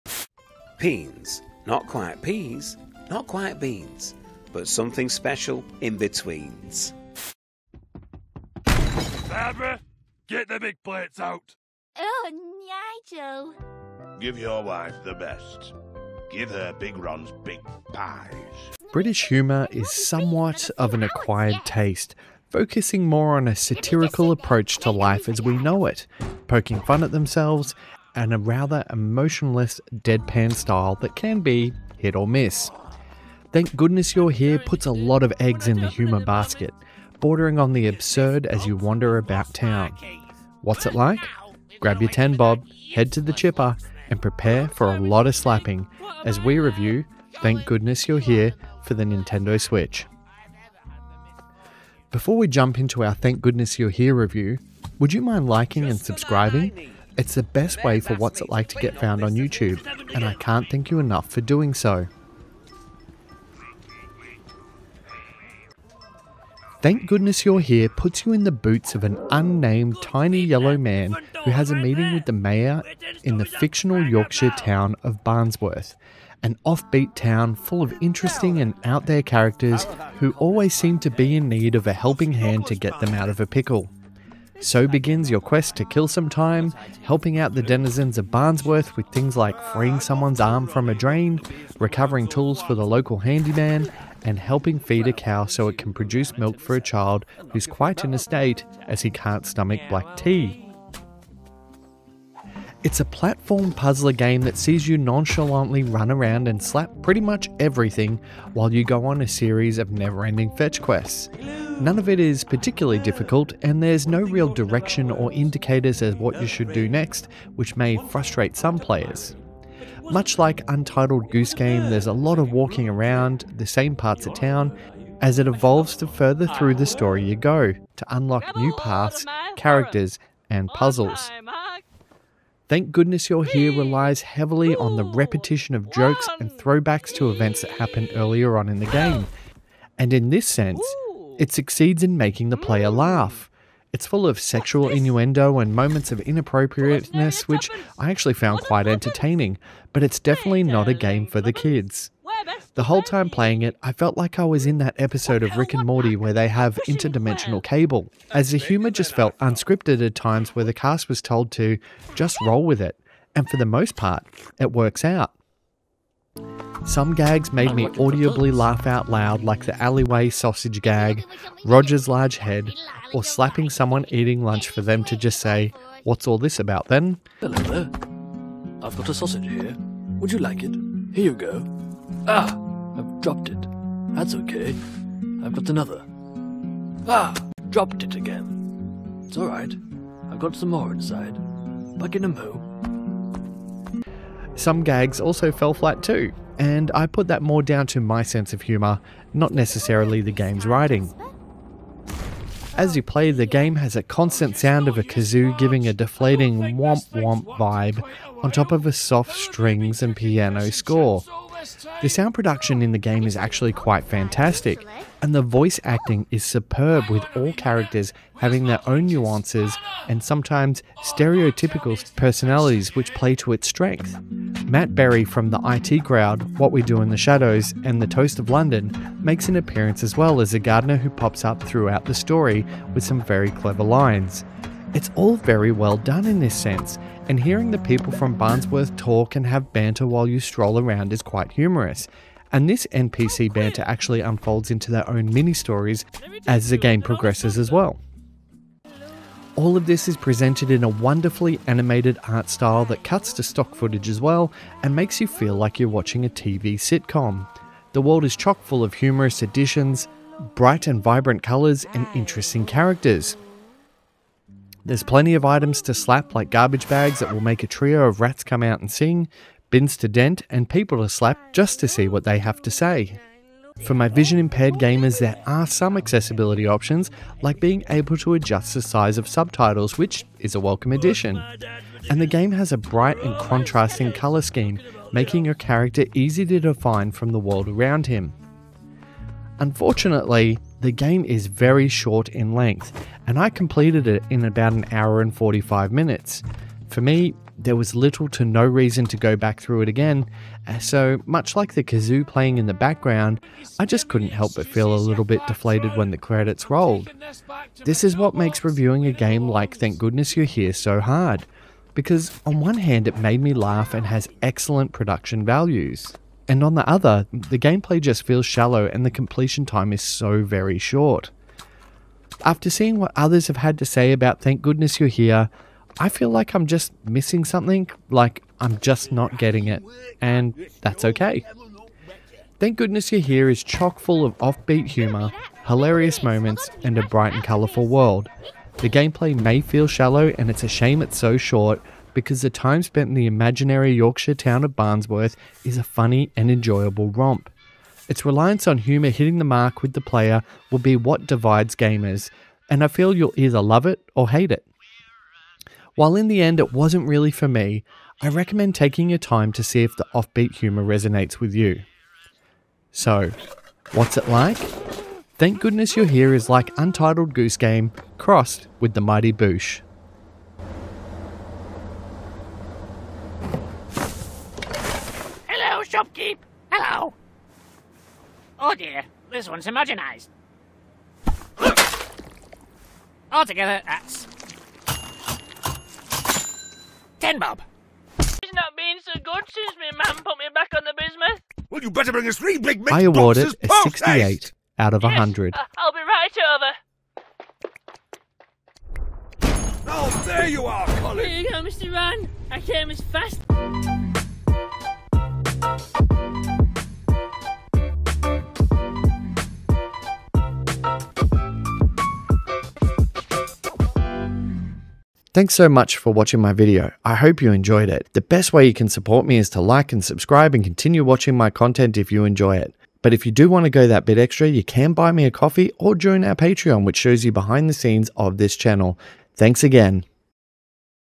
Thank Goodness You're Here Switch Review
British humour is somewhat of an acquired taste, focusing more on a satirical approach to life as we know it, poking fun at themselves, and a rather emotionless deadpan style that can be hit and miss. Thank Goodness You're Here puts a lot of eggs in the humour basket, bordering on the absurd as you wander about town. What's it like? Grab your ten bob, head to the chipper, and prepare for a lot of slapping, as we review Thank Goodness You're Here for Nintendo Switch